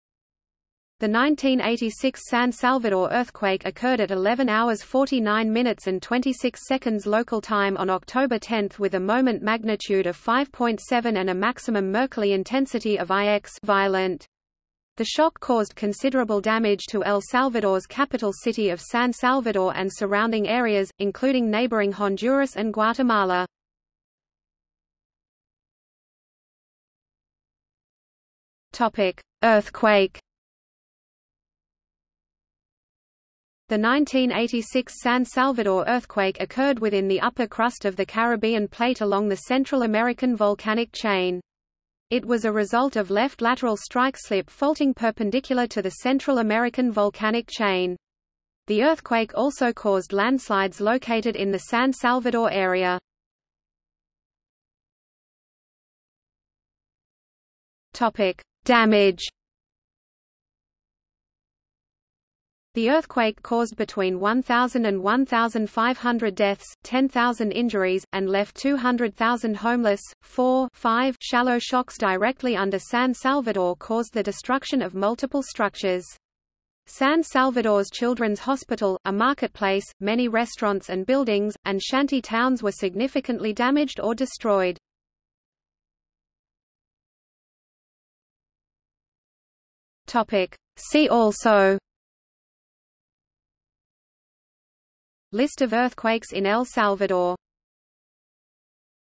1986 San Salvador earthquake | Wikipedia audio article
00:00:25 1 Earthquake
00:00:54 2 Damage
00:01:26 3 See also

- Socrates

SUMMARY
The 1986 San Salvador earthquake occurred at 11:49:26 local time on October 10 with a moment magnitude of 5.7 and a maximum Mercalli intensity of IX (Violent). The shock caused considerable damage to El Salvador's capital city of San Salvador and surrounding areas, including neighboring Honduras and Guatemala.